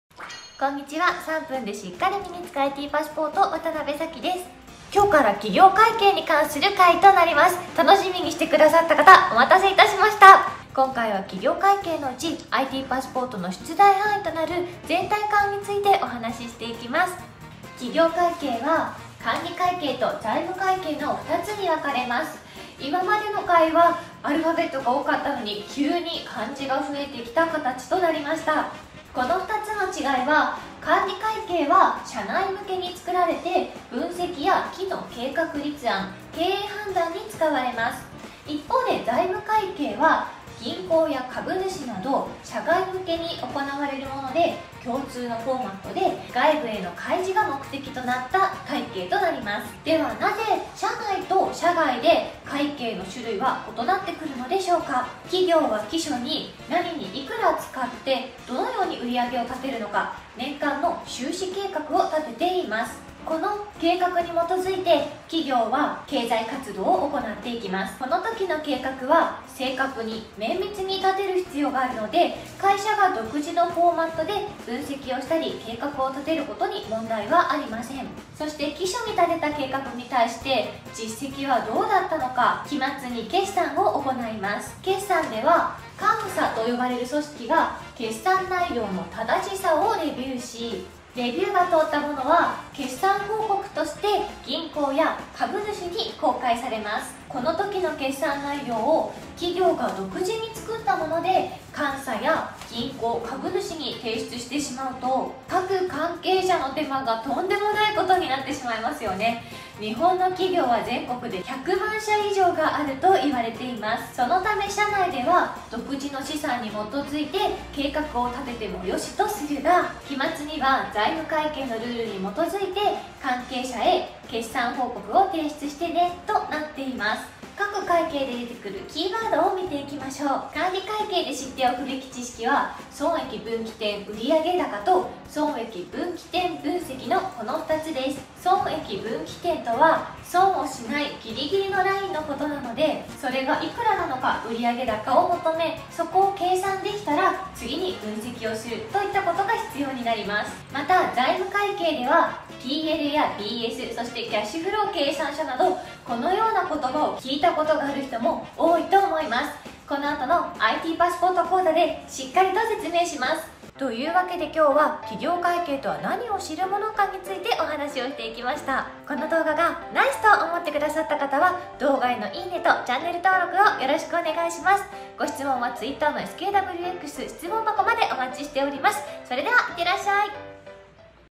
ITパスポート／企業会計の基礎知識
第24回は、企業会計がなんのためにあるものか、大枠について説明しています！細かいキーワードについては、第25回以降で解説します◎

▼復習Blogはこちら！

▼ITパスポート用語を3時間につぶやくTwitter bot